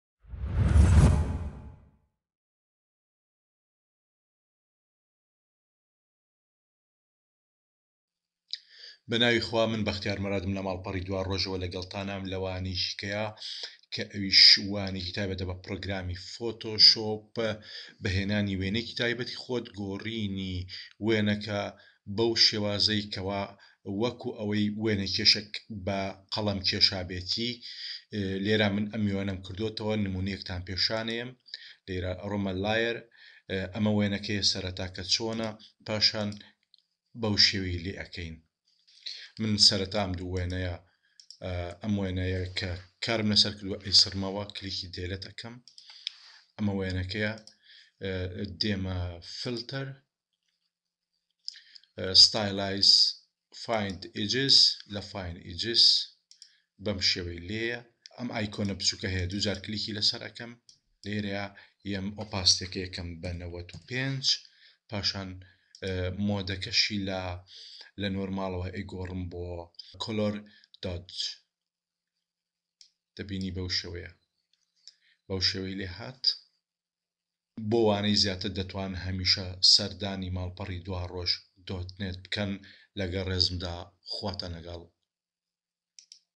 گۆڕێنی وێنە بەشێوازی وێنەکێش
لەم وانەیەدا باس لە چۆنیەتی گۆڕینی وێنەیەك دەکەین بۆ شێوازێك کە وەك ئەوەی وێنەکە بەدەست کێشرابێت لە لایەن وێنەكێشێکەوە، ئەویش بەبەکارهێنانی فلتەری ( فایند ئیجز) لە
پرۆگرامی فۆتۆشۆپ دا،بەهیواین کە وانەکە سوودمەند بێت.
لە گەڵ ڕێزم دا.